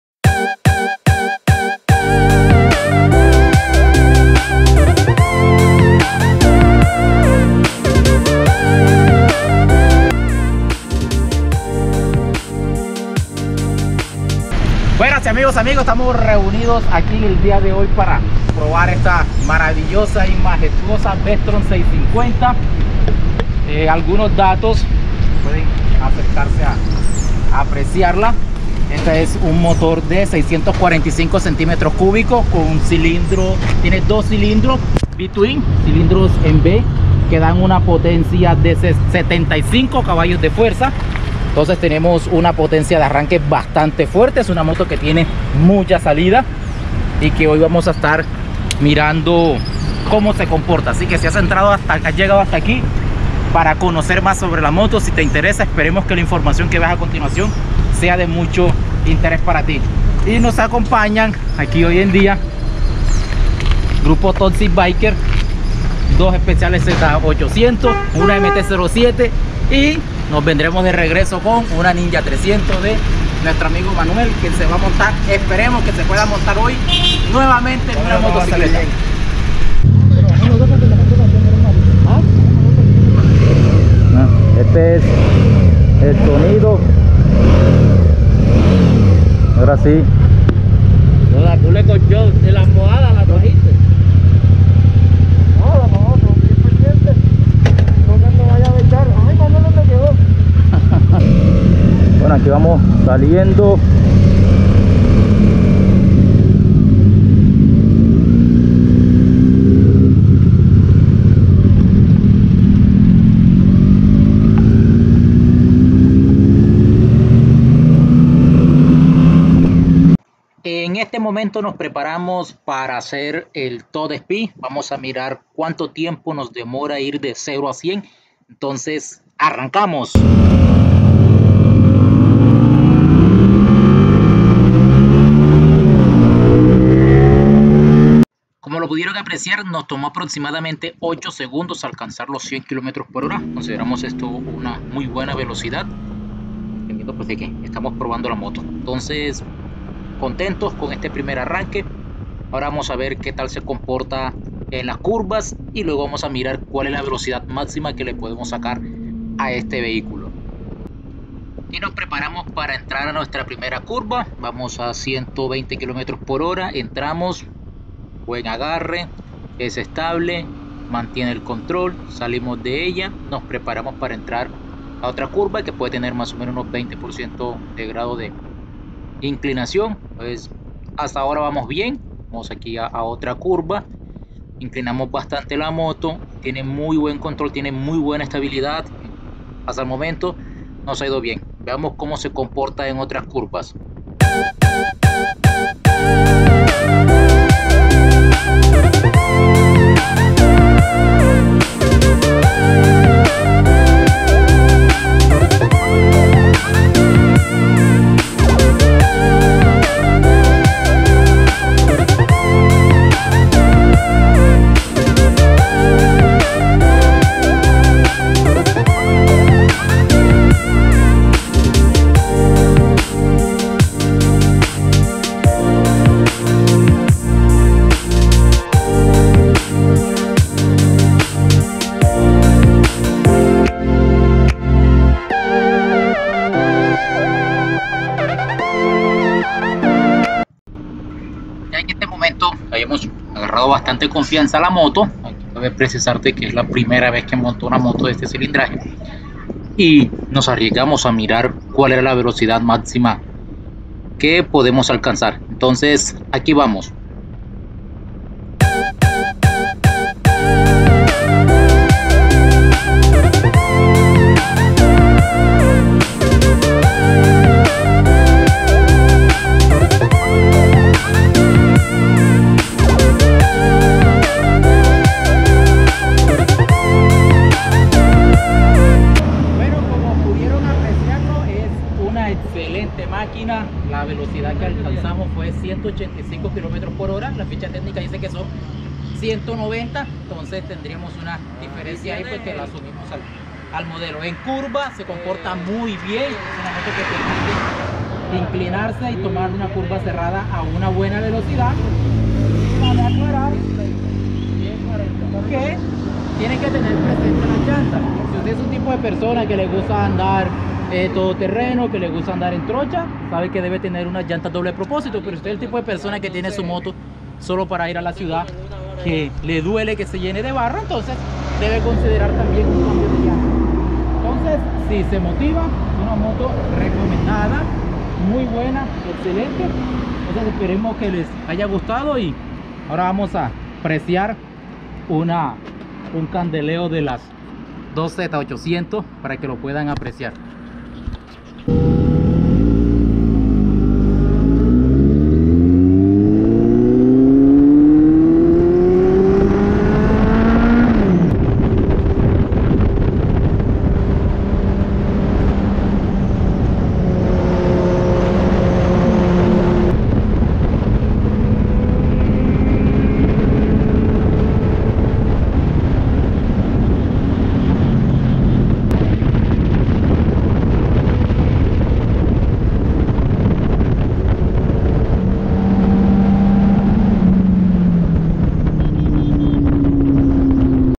Primera Rodada en mi V Strom 650
Descripción de algunos detalles técnicos de una Suzuki V Strom 650
Detalles sobre como se comporta en curvas y velocidad final